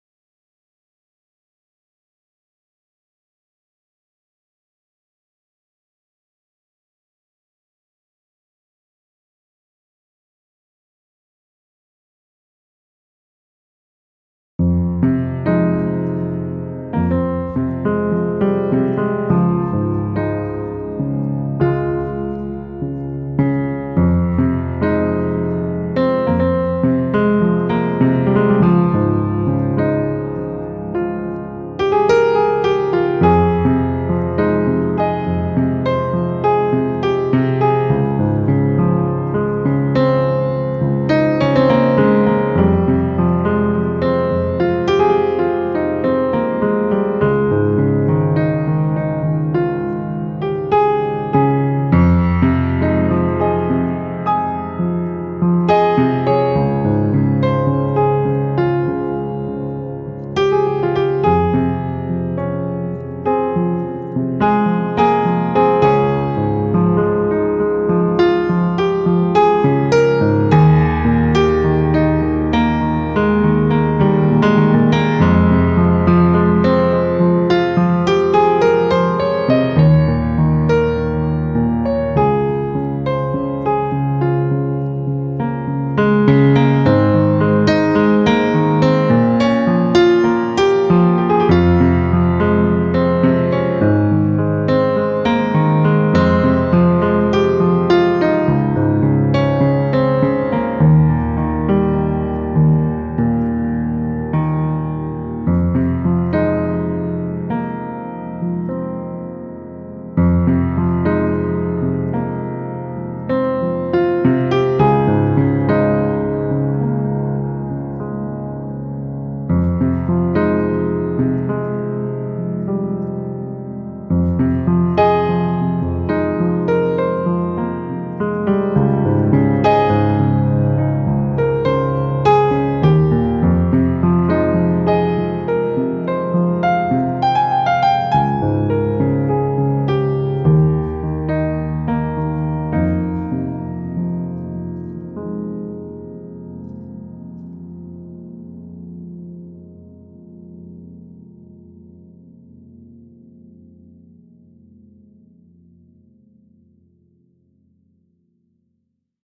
Piano improvisation
Mixed with Maschine
With Korg M3 , guitars, instruments and my little hands
Musician Videira Rémi
Mix Videira Rémi